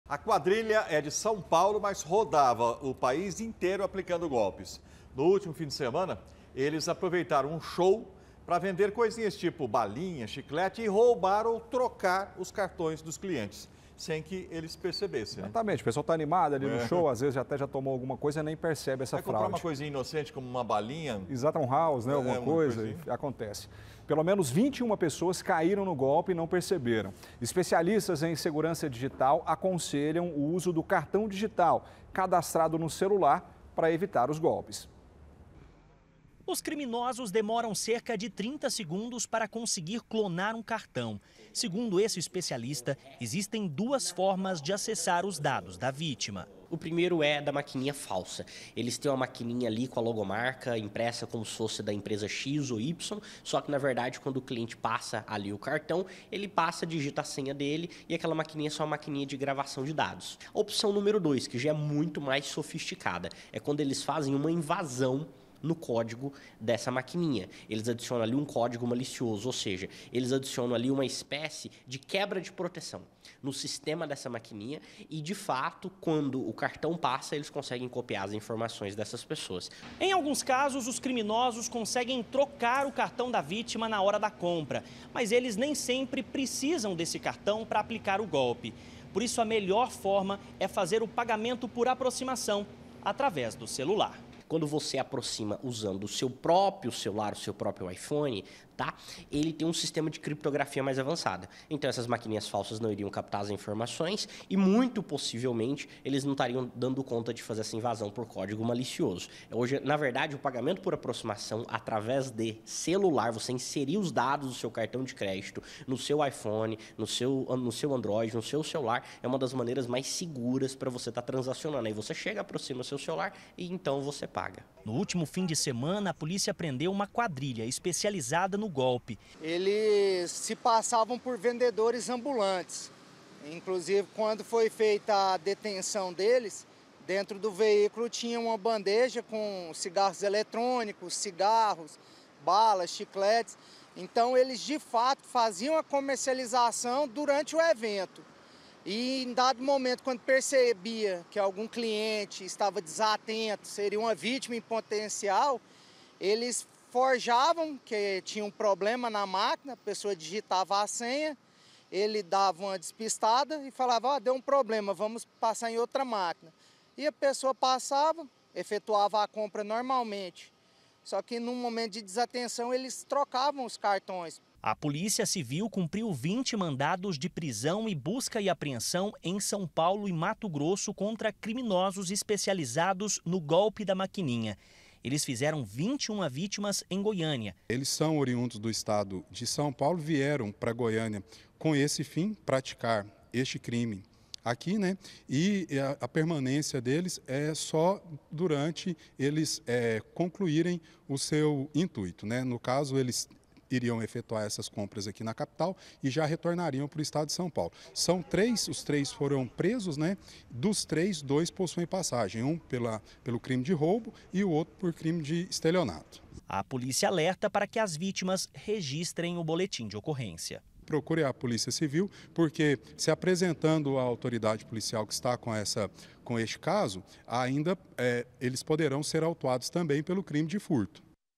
GOLPE DA MAQUININHA: CRIMINOSOS TROCAVAM CARTÕES DE CLIENTES EM SHOW
PRESA A QUADRILHA QUE APLICAVA O FAMOSO GOLPE DA MAQUININHA, EM GOIÂNIA. A QUADRILHA É DE SÃO PAULO, MAS RODAVA O PAÍS APLICANDO GOLPES. NO ÚLTIMO FIM DE SEMANA, ELES APROVEITARAM UM SHOW PARA VENDER COISINHAS COMO BALINHAS E CHICLETES E ROUBAR OU  TROCAR OS CARTÕES DOS CLIENTES, SEM QUE ELES NOTASSEM.